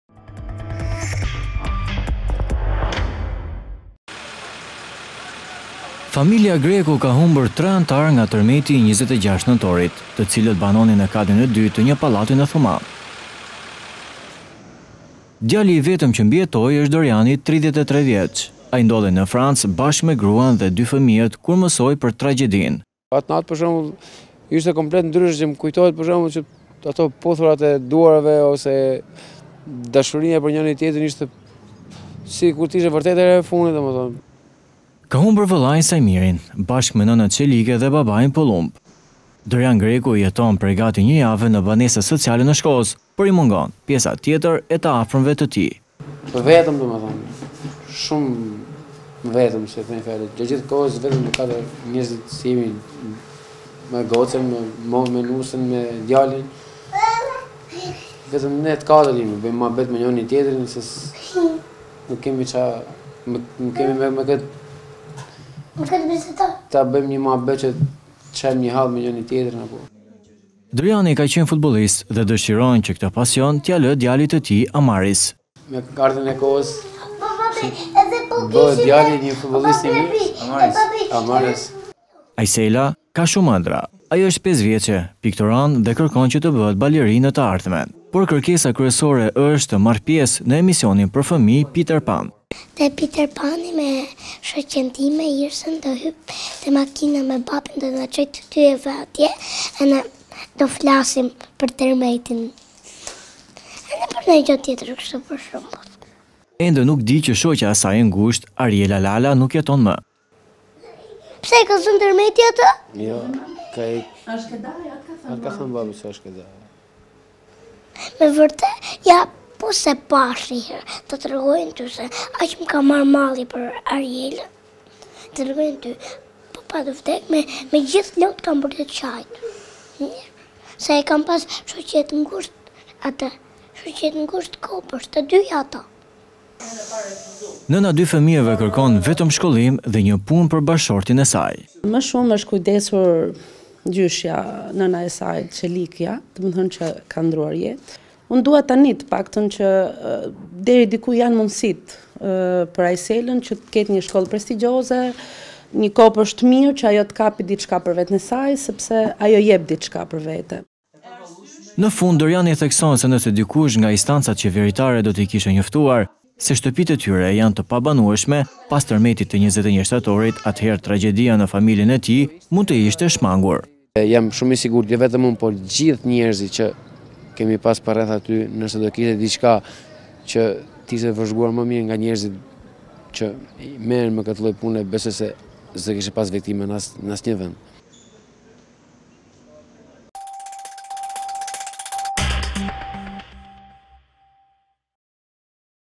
Familja Greku, Dorjani: Tragjedia mund te shmangej pas termetit te 21 shtatorit | ABC News Albania
Katër familje që humbën njerëzit e tyre të dashur në Thumanë pas tërmetit të 26 nëntorit janë strehuar në banesat sociale në Shkozë. Abc News ishte pranë familjes Greku, që humbi 3 anëtar.

Dorjani është kthyer nga Franca për t’i qëndruar pranë familjes. Ai thotë se tragjedia mund të ishte shmangur nëse do të ishin njoftuar se banesa e familjes së tij në Thumanë ishte e pabanueshme që pas tërmetit të 21 shtatorit.

Familja Greku ka humbur 3 anëtar nga tërmeti i 26 nëntorit, të cilët banonin në katin e dytë të një pallati në Thumanë. Djali i vetëm që mbijetoi është Dorjani 33-vjeç. Ai ndodhej në Francë bashkë me  gruan dhe dy fëmijët ku mësoi për tragjedinë.

Ka humbur vëllain e Saimirin bashkë me nënën Çelike dhe babain Pëllumb. Dorjani Greku jeton prej gati një jave në banesat sociale në Shkozë por i mungon pjesa tjetër e të afërmve të tij.

Dorjani dikur ka qenë futbollist dhe dëshiron që këtë pasion t’ia lë djalit të tij, Amaris.

Ajsela ka shumë ëndrra… ajo është 5 vjeçe, pikturon dhe kërkon që bëhet  balerinë në të ardhmen. Por kërkesa kryesore është të marr pjesë në emisionin “Piter Pan”.

Ende nuk e di që shoqja e saj e ngushtë Ariela Lala nuk jeton më.

Nëna e dy fëmijëve kërkon vetëm shkollim dhe një punë për bashkëshortin e saj.

Në fund Dorjani thekson se nëse dikush nga instancat qeveritare do ti kishte njoftuar se banesa e tyre ishte e pabanueshme pas tërmetit të 21 shtatorit atëherë tragjedia në familjen e tij mund të ishte shmangur./abcnews.al